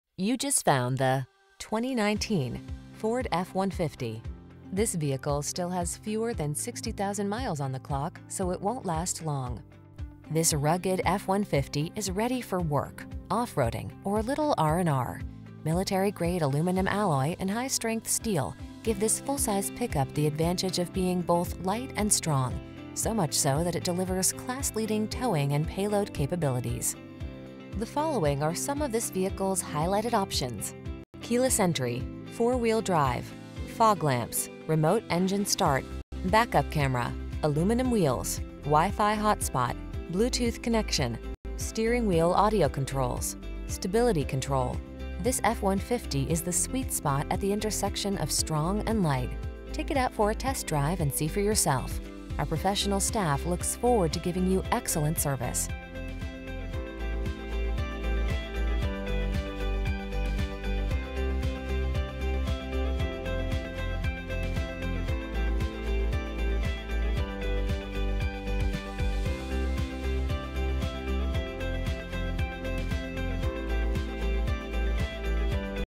2019 Ford F-150 Rochester, Victor, Pittsford, Webster, Spencerport, NY VT42793
Oxford White Used 2019 Ford F-150 available in Rochester, New York at Auction Direct USA Rochester. Servicing the Victor, Pittsford, Webster, Spencerport, NY area.

4WD! Bluetooth, Remote Keyless Entry, Steering Wheel Mounted Audio Controls, USB Port, Alloy Wheels. This vehicle has a clean, one-owner CARFAX history report.